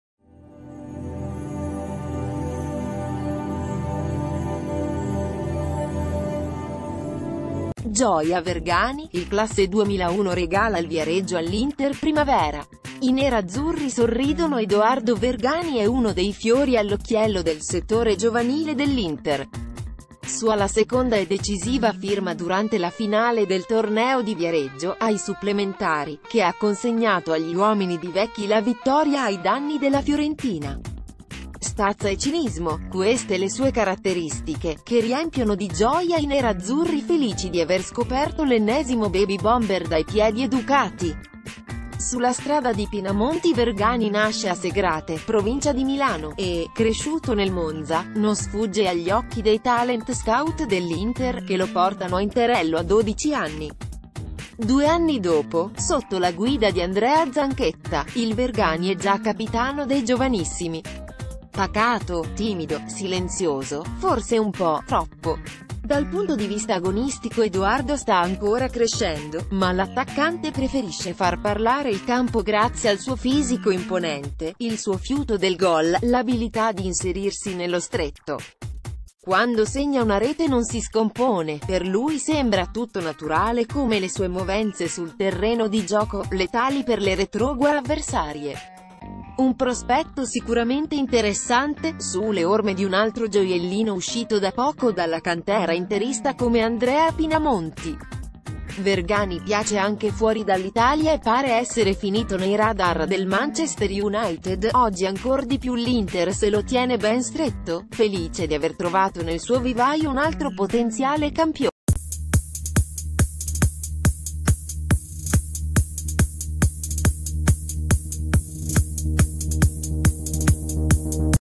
Ultime notizie | Inter, Vergani sulle orme di Pinamonti. Ecco il diciassettenne che ha deciso il ...
Ultime notizie | Inter, Vergani sulle orme di Pinamonti. Ecco il diciassettenne che ha deciso il Viareggio

Gioia Vergani: il classe 2001 regala il Viareggio all’Inter Primavera. I nerazzurri sorridono Edoardo Vergani è uno dei fiori all’occhiello del settore giovanile dell’Inter. Sua la seconda e decisiva firma durante la finale del Torneo di Viareggio (ai supplementari), che ha consegnato agli uomini di Vecchi la vittoria ai danni della Fiorentina. Stazza e cinismo: queste le sue caratteristiche, che riempiono di gioia i nerazzurri felici di aver scoperto l’ennesimo baby-bomber dai piedi educati. SU...